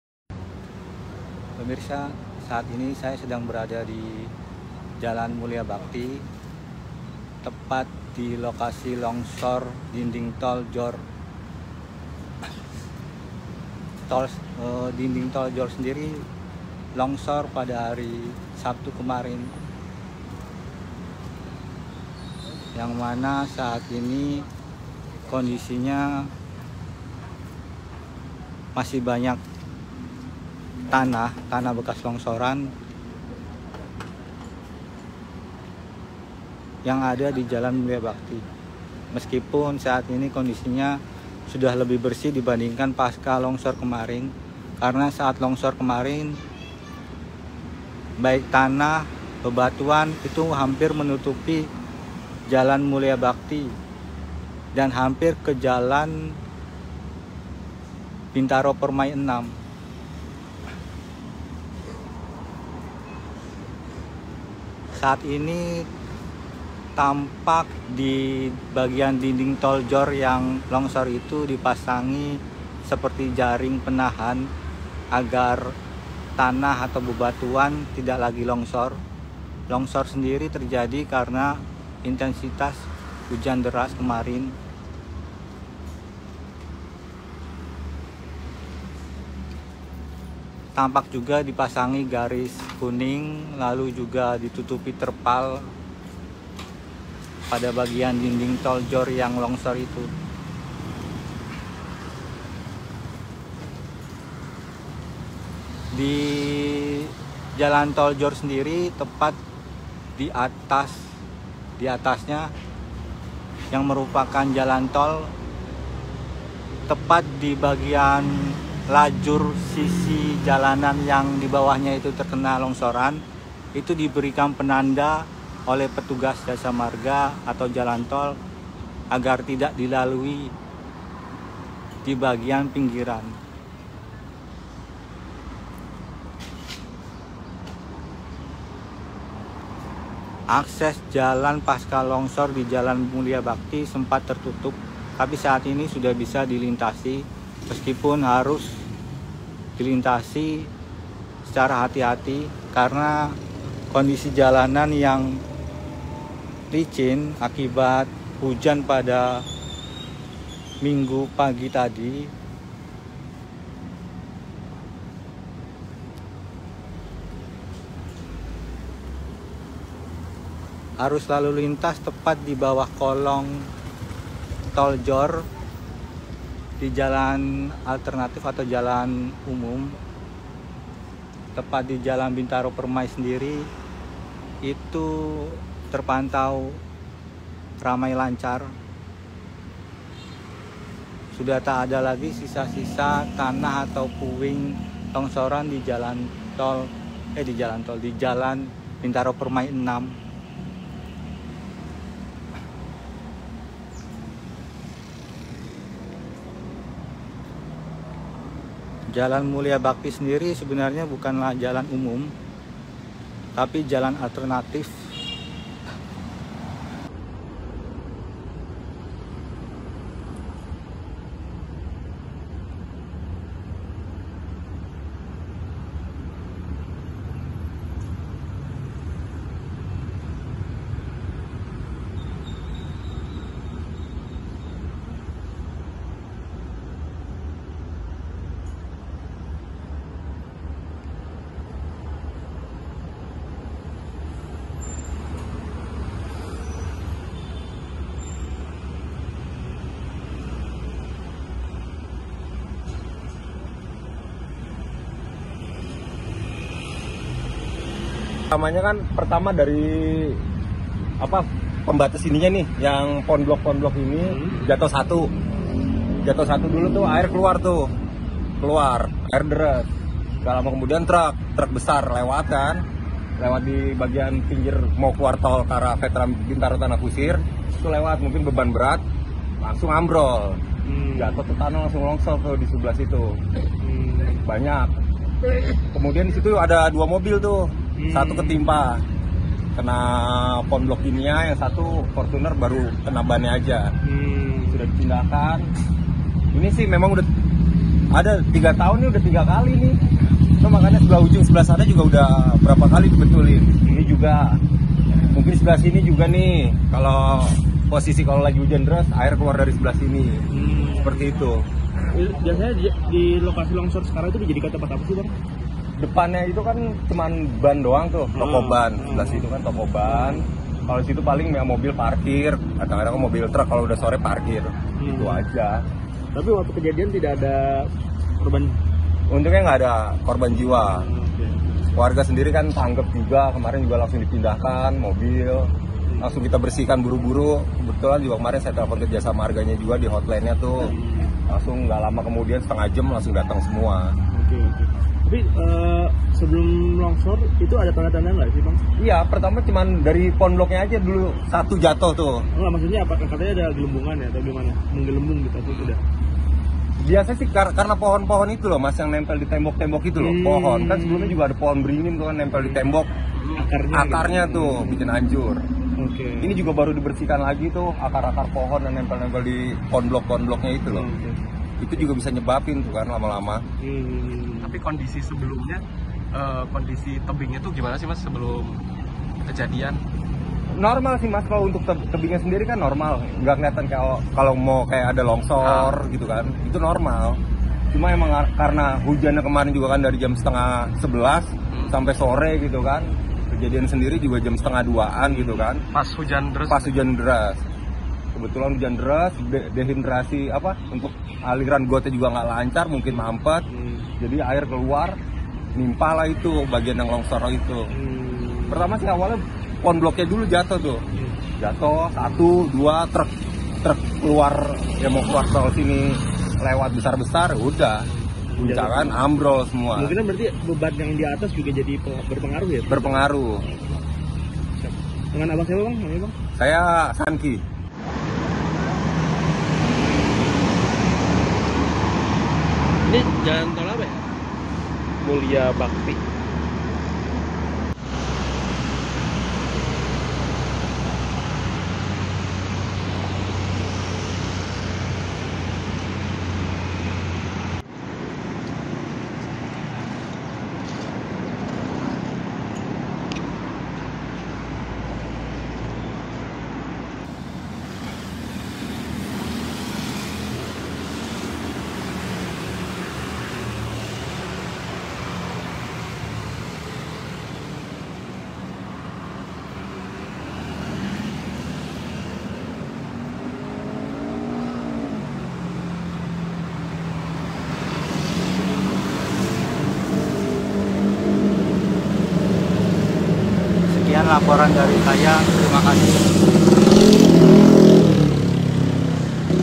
[FULL] Penampakan Jalur Tol JORR Pasca Longsor Kemarin: Masih Banyak Tanah
Warga Jalan Mulia Bhakti berharap agar Jasa Marga atau pihak pengelola Jalan Tol JORR memperbaiki dan memperkuat dinding yang longsor pada Sabtu, 6 Juli 2024 kemarin. Pasalnya, dinding jalur tol tersebut sudah 3 kali ambrol.

"Saya harap bisa diperbaiki dengan benar, hati-hati biar gak ambrol lagi, dan jangan sampai ada korban. Karena ini kan sebenarnya sudah 3 kali kejadian serupa di barisan sini," ujar Sankti pada wartawan, Minggu (7/7/2024).